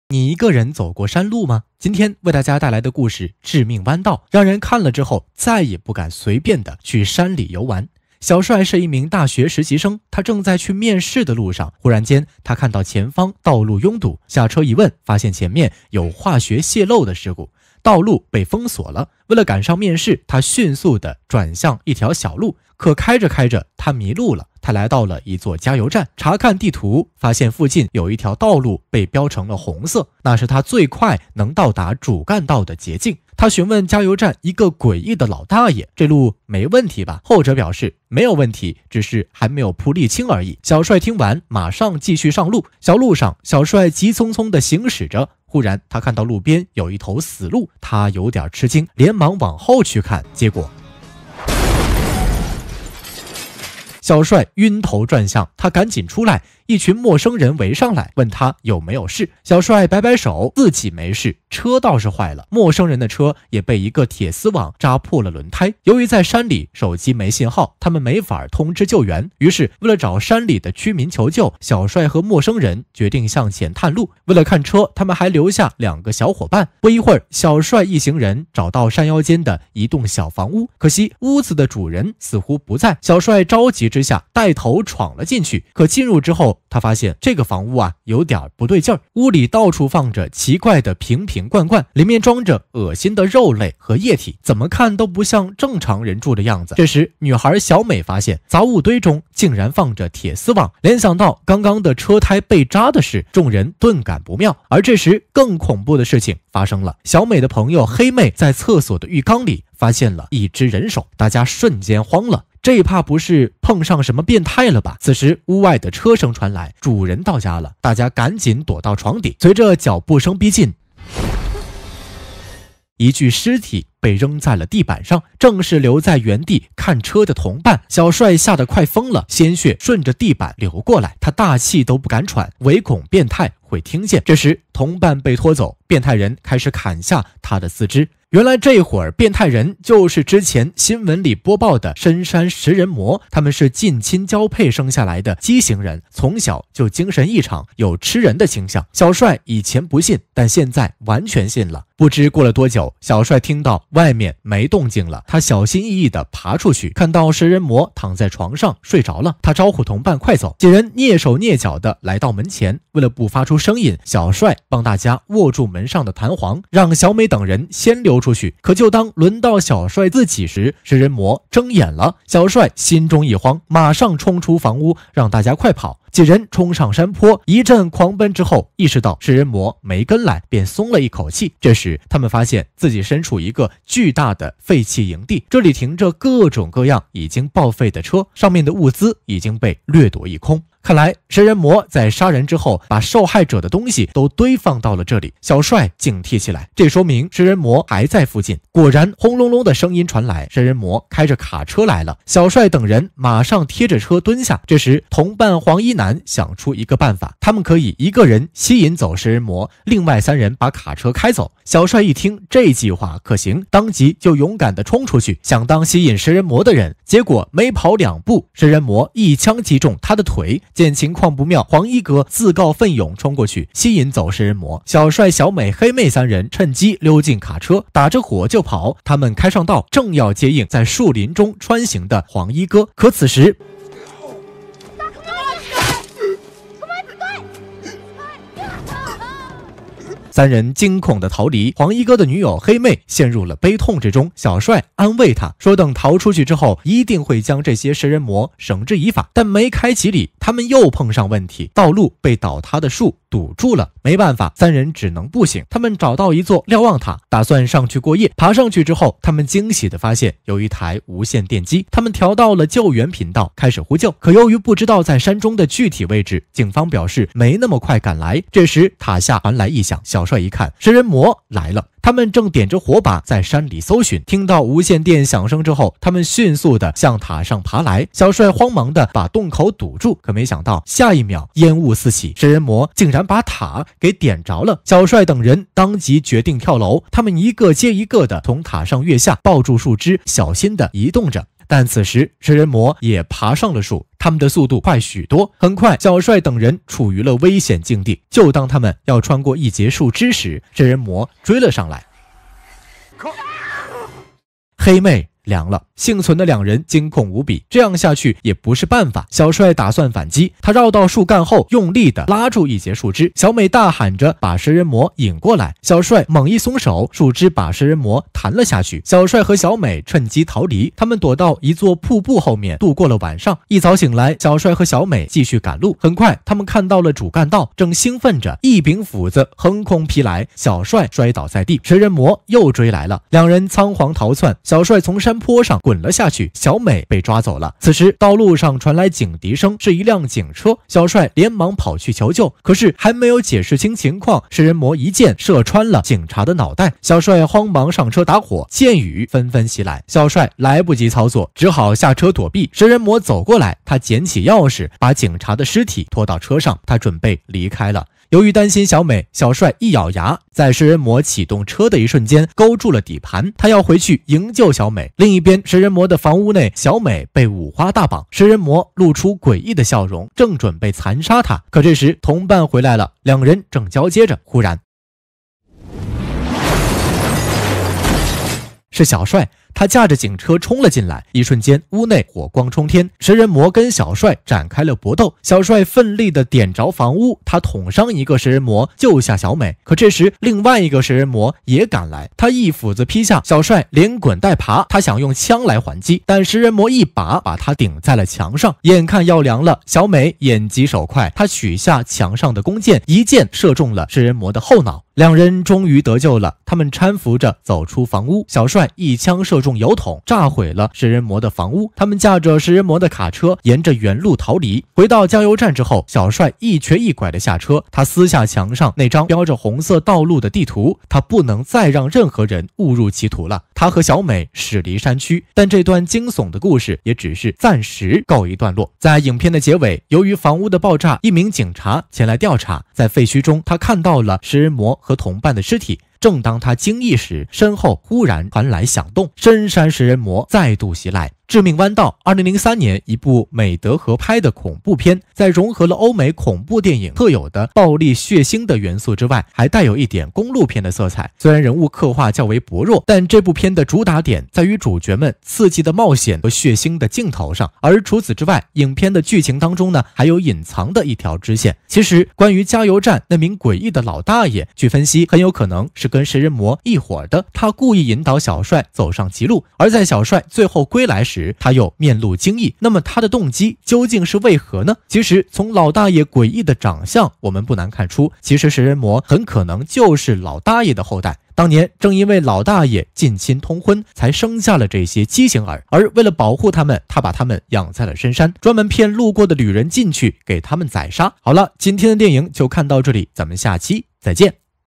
实习生为赶时间开进小路，却不知等待自己的是什么，冒险电影
加油站大爷与食人族为伍，专门骗路人进入圈套